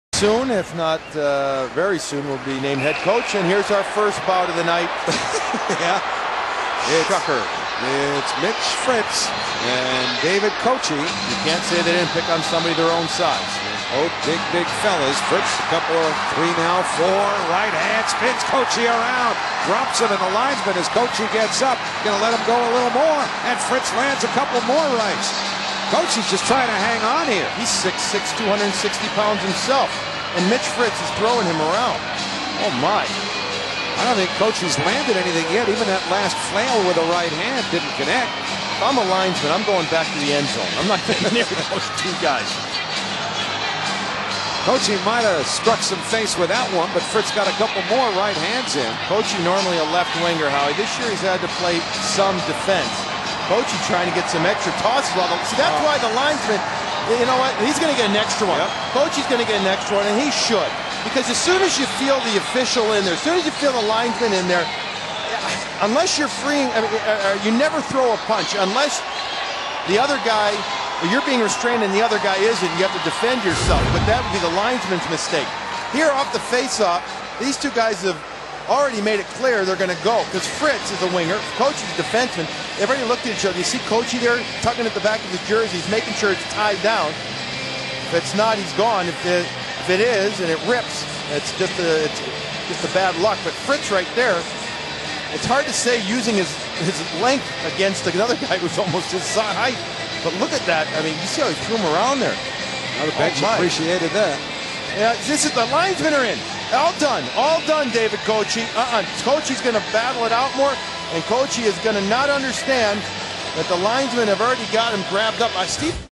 David Koci vs Mitch Fritz Apr 4, 2009
David Koci vs Mitch Fritz from the Tampa Bay Lightning at New York Islanders game on Apr 4, 2009.